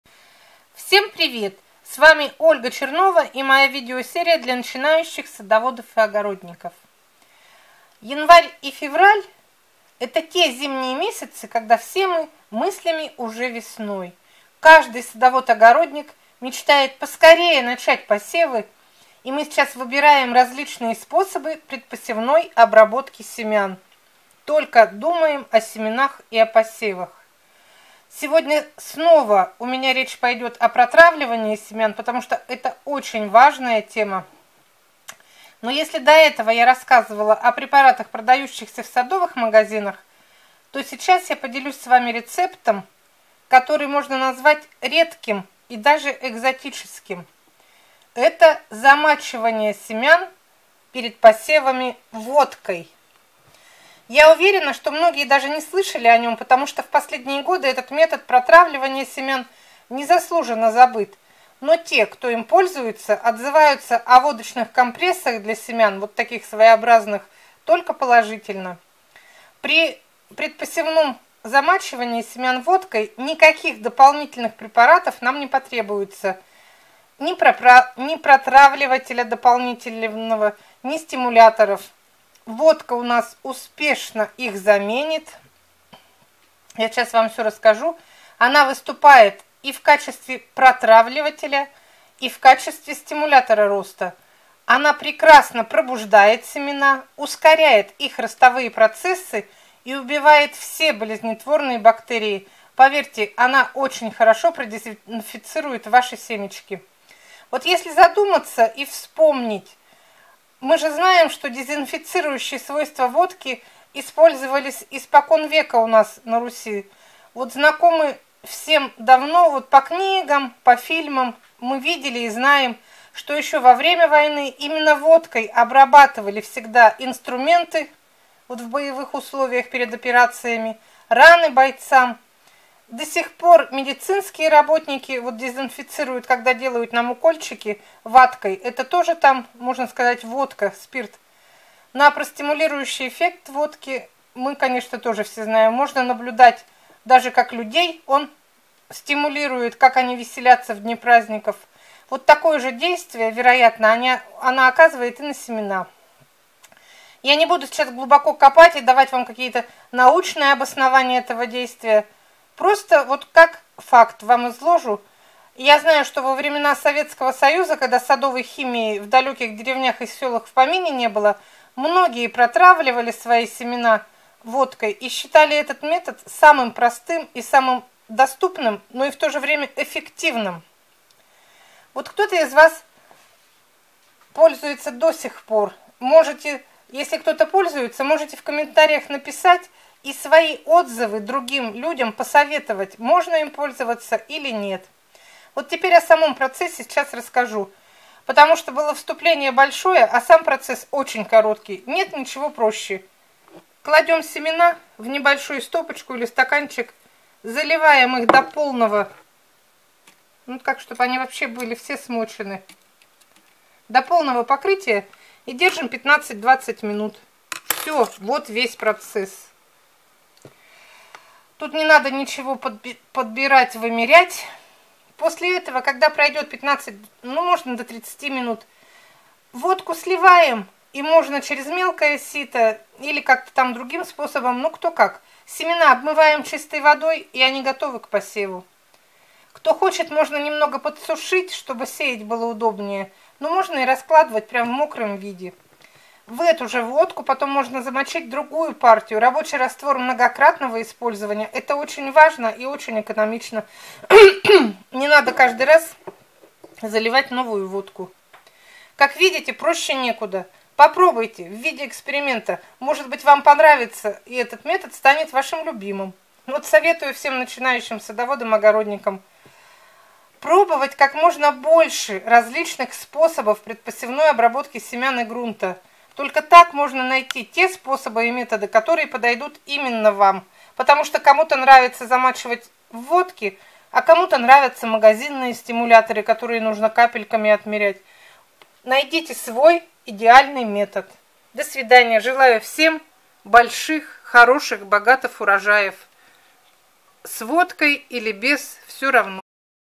САМЫЙ БЫСТРЫЙ И ЭФФЕКТИВНЫЙ СПОСОБ ОБЕЗЗАРАЖИВАНИЯ СЕМЯН ПЕРЕД ПОСЕВАМИ. ВОДКА. Ольга Чернова.
Если до этого я описывала самые распространенные методы дезинфекции семян  - замачивание в Фитоспорине,  протравливание марганцовкой, то этот метод  - замачивание в водке - очень редкий. Но очень простой и эффективный.